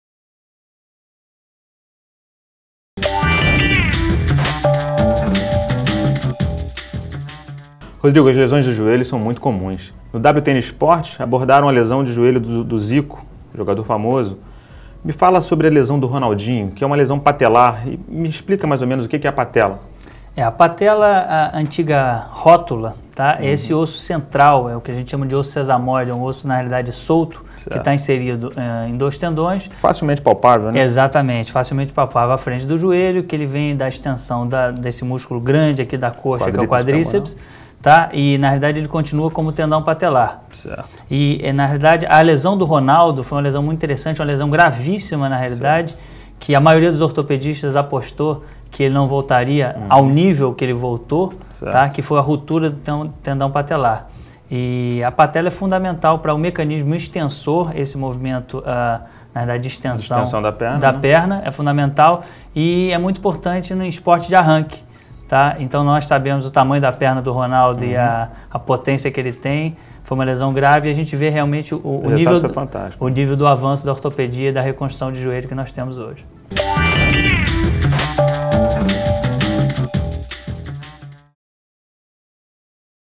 WTN SAÚDE DR. RODRIGO KAZ MEDICINA DO ESPORTE (EXTRA 1)
DR. RODRIGO KAZ EXPLICA SOBRE MEDICINA DO ESPORTE.